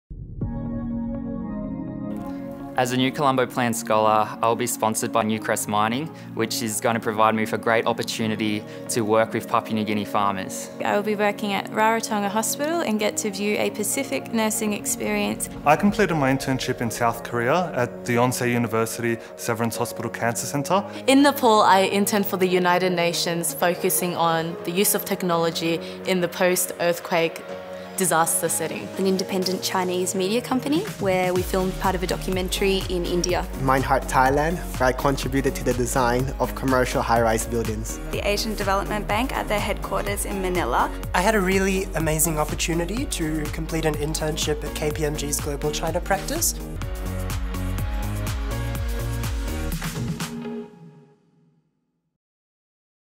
What internship opportunities are there with the New Colombo Plan?
What internship opportunities are there with the New Colombo Plan?  2019 Scholars and NCP alumni answer the question.